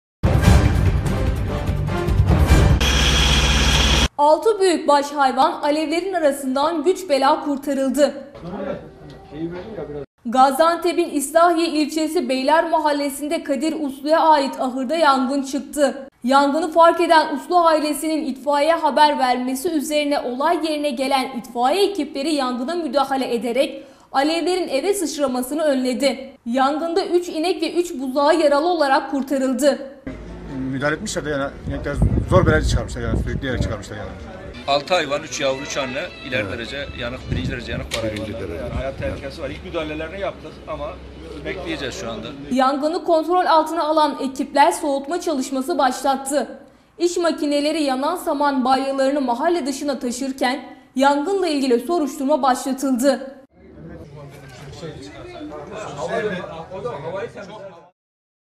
6 BÜYÜKBAŞ HAYVAN ALEVLERİN ARASINDA KALDI
İslahiye’de bir ahırda henüz bilinmeyen bir nedenle yangın çıktı. 6 büyükbaş hayvan, alevlerin arasından yaralı olarak kurtarıldı.
6 büyükbaş hayvan alevlerin arasından güç bela kurtarıldı.
Gaziantep’in İslahiye ilçesi Beyler Mahallesi’nde Kadir Uslu'ya ait ahırda yangın çıktı. Yangını fark eden Uslu ailesinin, itfaiyeye haber vermesi üzerine olay yerine gelen itfaiye ekipleri yangına müdahale ederek, alevlerin eve sıçramasını önledi. Yangında 3 inek ve 3 buzağı yaralı olarak kurtarıldı.
Yangını kontrol altına alan ekipler, soğutma çalışması başlattı. İş makineleri, yanan saman balyalarını mahalle dışına taşırken yangınla ilgili soruşturma başlatıldı.